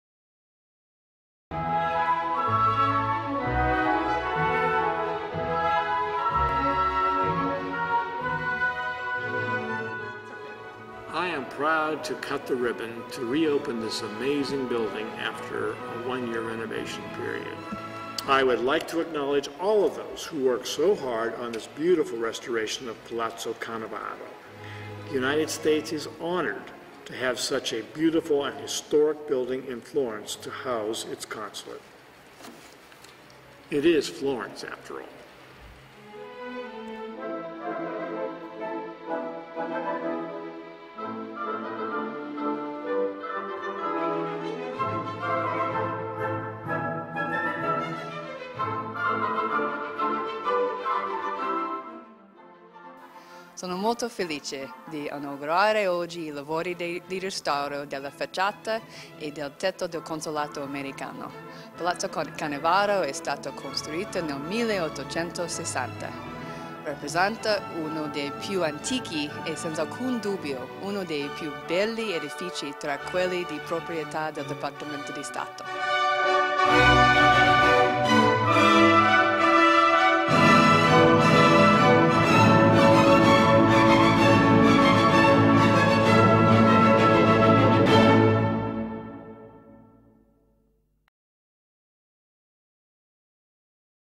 Il Consolato USA di Firenze restaurato.
Cerimonia di inaugurazione dei lavori di restauro della facciata e del tetto di Palazzo Canevaro, l’edificio che ospita il Consolato generale americano di Firenze.

L’Ambasciatore degli Stati Uniti John Phillips ha tagliato il nastro, alla presenza del Console Generale Abigail Rupp, del Vice Direttore del Bureau of Overseas Building Operations del Dipartimento di Stato, Heather Townsend, del Presidente della Regione Toscana Enrico Rossi, dell'Assessore all’Urbanistica e Politiche del Territorio Elisabetta Meucci, oltre ad autorità locali, esponenti della società civile e rappresentanti della stampa.

“E’ un onore per gli Stati Uniti avere un edificio di tale pregio storico come sede del Consolato”, ha sottolineato l’Ambasciatore Phillips, durante la cerimonia del taglio del nastro. Il Console Rupp ha, dal canto suo, ricordato che Palazzo Canevaro “è stato costruito nel 1860 e rappresenta uno dei più antichi e senza dubbio uno dei più belli edifici tra quelli di proprietà del Dipartimento di Stato.”

Per maggiori informazioni sulle attività del Bureau of Overseas Building Operations, clicca qui